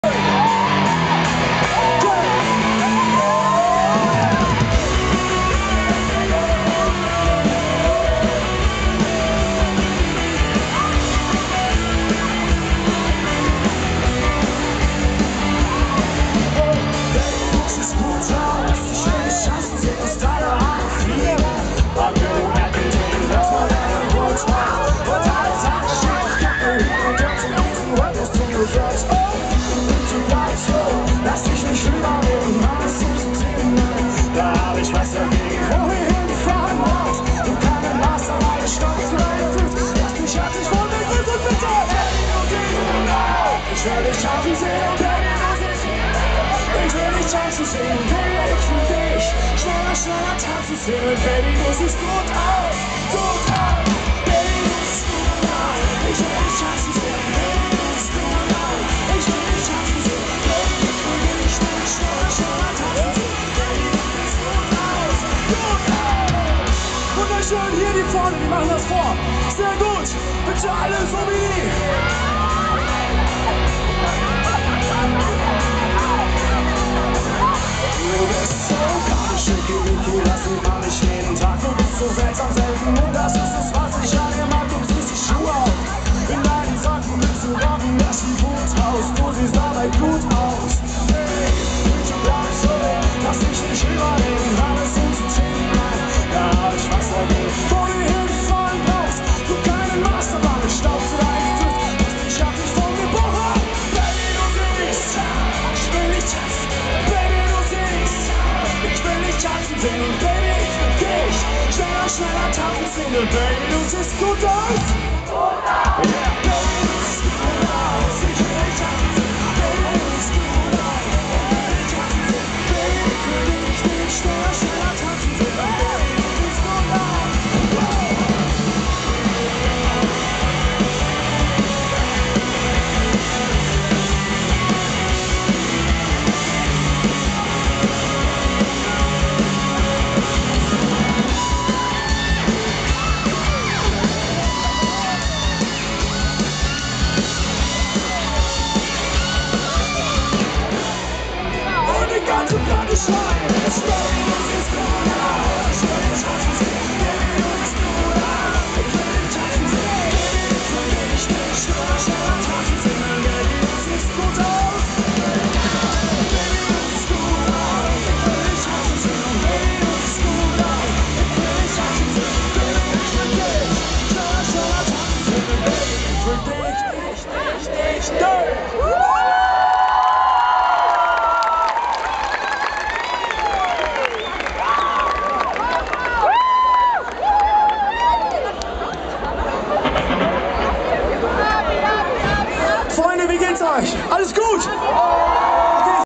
BAKKUSHAN (live) @ BOCHUM TOTAL 2012
Bakkushan machten die Kreuzung vor der ''1Live''Bühne zur PartyMeile! :)
''Bochum sah gut aus''!